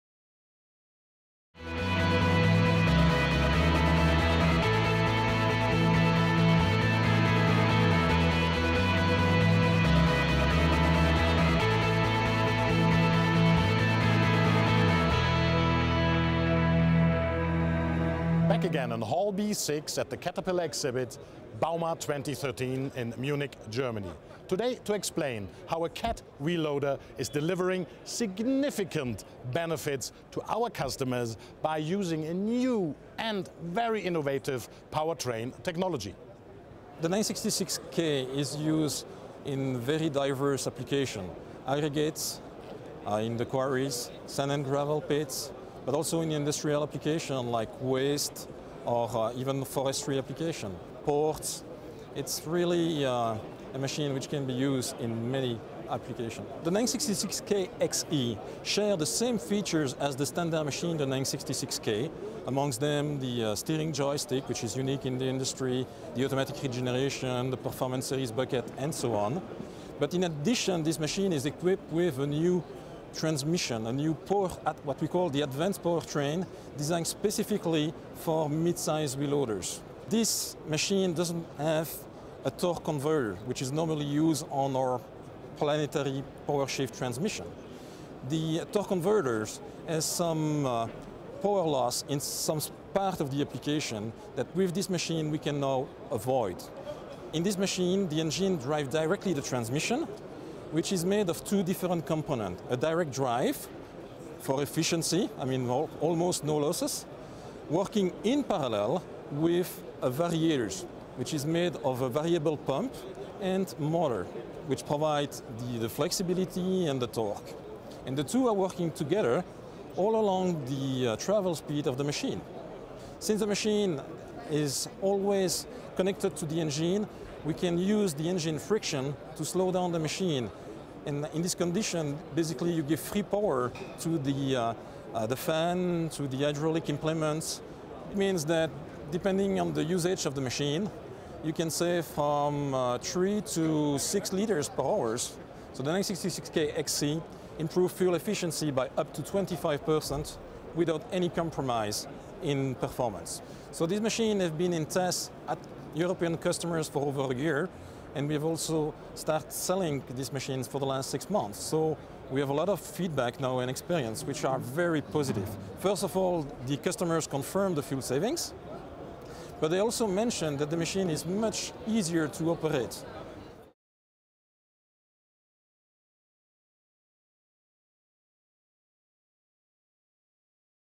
A Look at the Cat® 966K XE Wheel Loader | bauma Munich
The Cat® 966K XE Wheel Loader with advanced powertrain delivers up to 25% increase in fuel efficiency (tons/liter). It's one of many Cat products driving fuel efficiency through innovation. Visit us in Hall B6 at bauma Munich, April 15-21, 2013, to see this and other Cat machines on display.